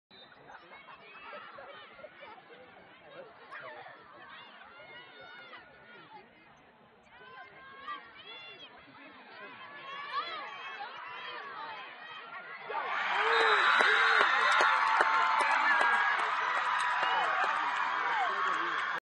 GV: Ana Hagerup goal
GV: Ana Hagerup goal - High School Girls Lacrosse: Georgetown Visitation vs. St. Stephen's & St. Agnes (4/18/2012)